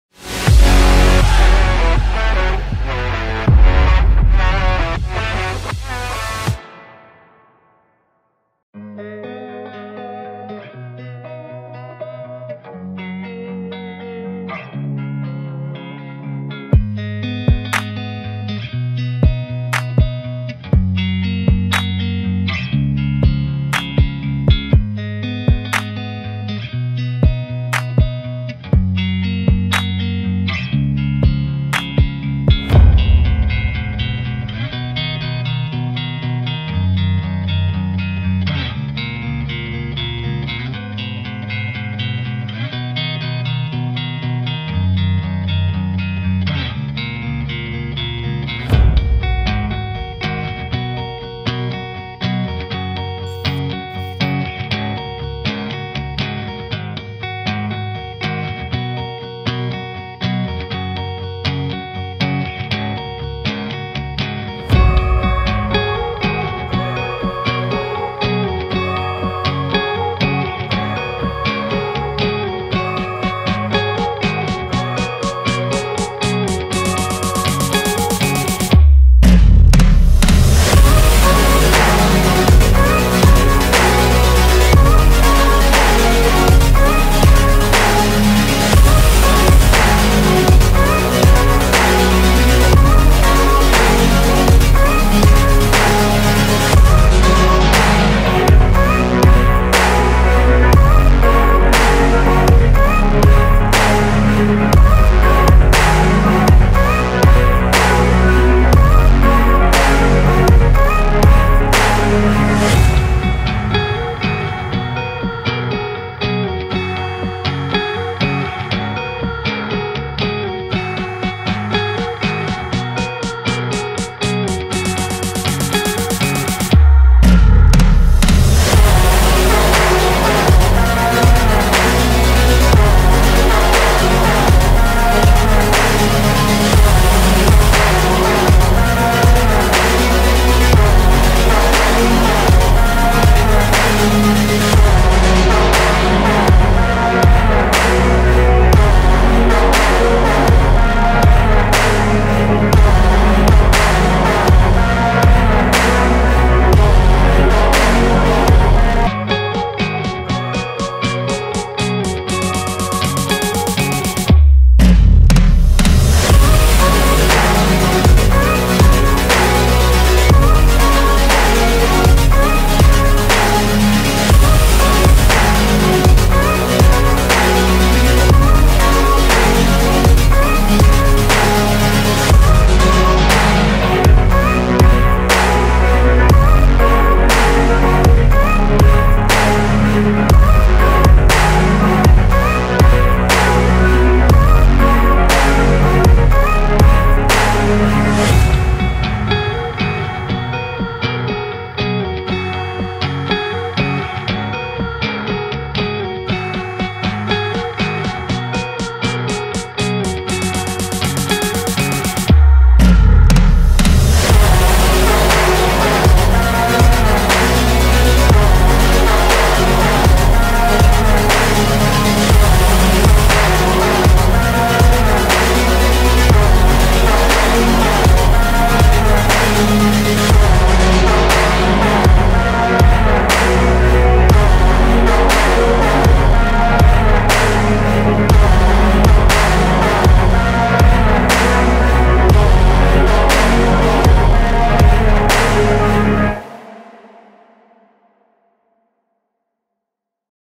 Renault Kangoo 1.5 DCI ZEN 2018R. | APS SIEDLCE
Prezentacja najnowszej oferty dealera samochodów używanych Auta Poleasingowe Siedlce, zapraszamy do obejrzenia oraz do odwiedzenia naszej strony